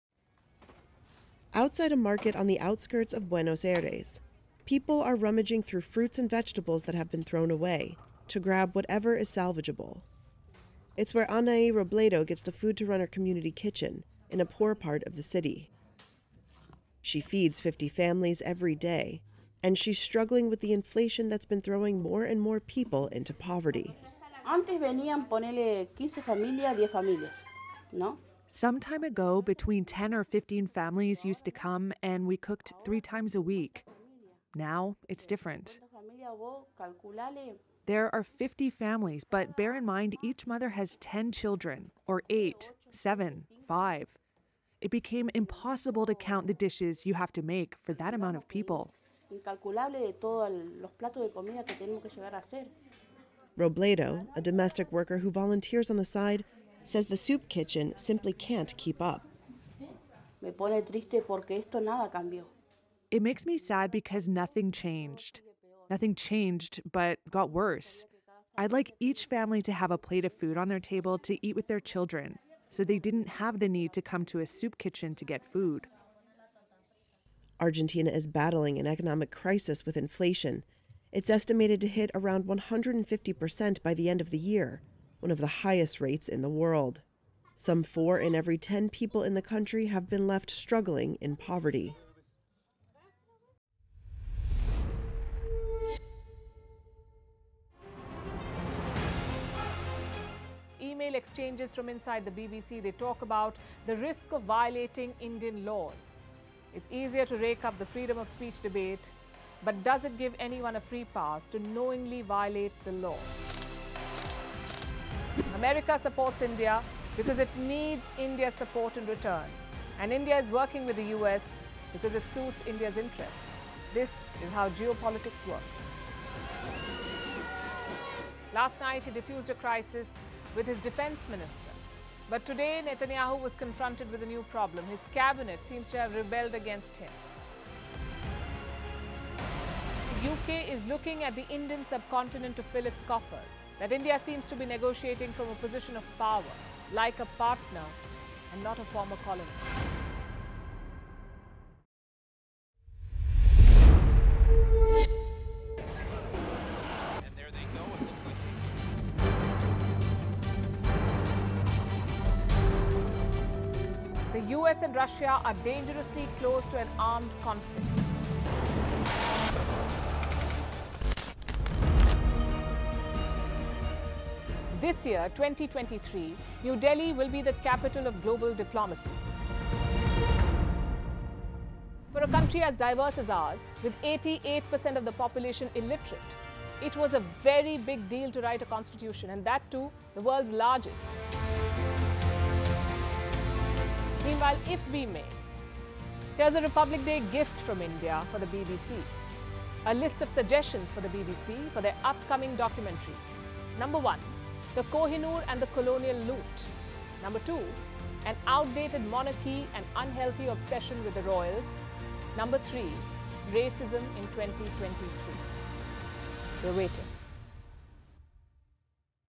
Demand for Soup Kitchens Rises in Argentina
Rising inflation levels, almost expected to touch 150% by the year end, has paved a dramatic rise in demand for soup kitchens in Argentina. Anahí Robledo, who runs a community kitchen in Buenos Aires, says she is struggling to feed the 50 families that rely on her kitchen, and is distressed with the rising inflation that is throwing majority of people into poverty.

 Firstpost | World News | Latest News